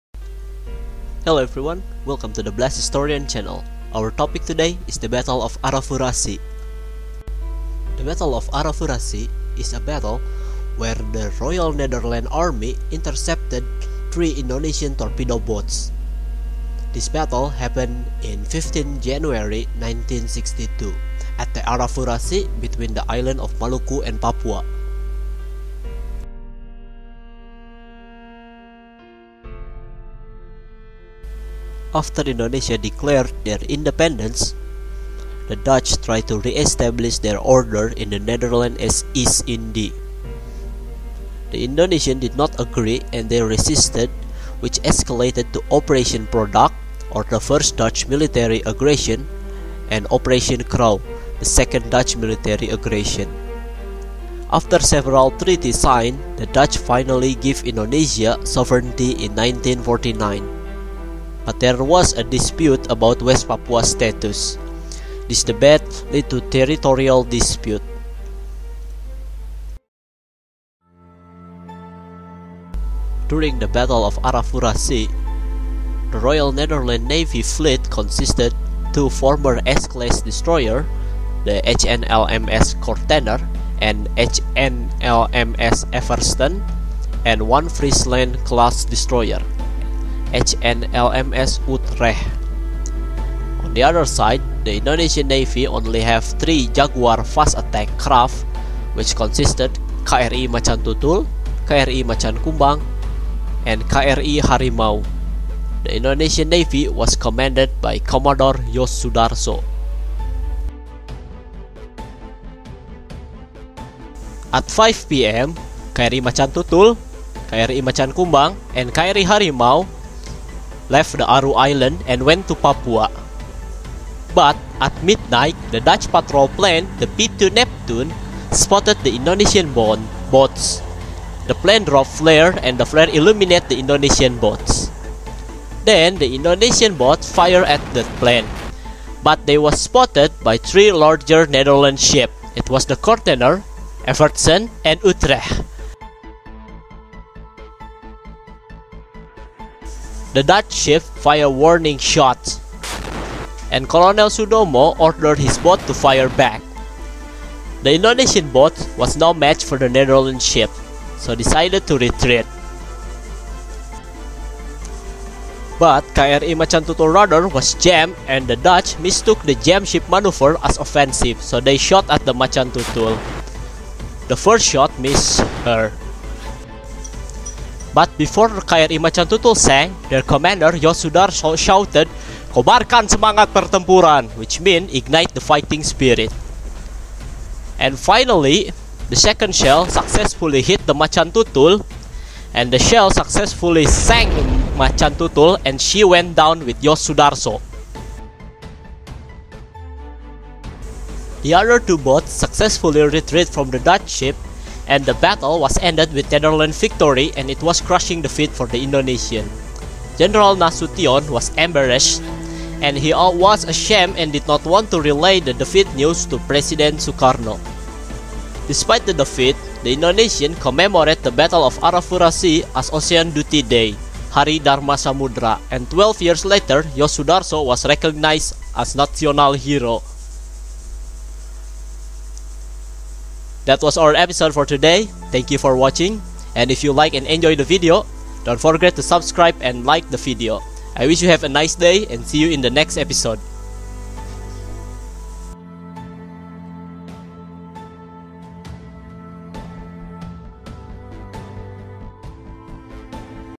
The Battle of Arafura Sea Brief Explanation
Hello Everyone welcome to the Blast Historian Channel, our topic today is the battle of Arafura Sea.The Battle of Arafura Sea is a battle where the Royal Netherland Navy intercepted 3 Indonesian Torpedo boats. This Battle happened 15 January 1962 at the Arafura sea,  between islands of Maluku and Papua.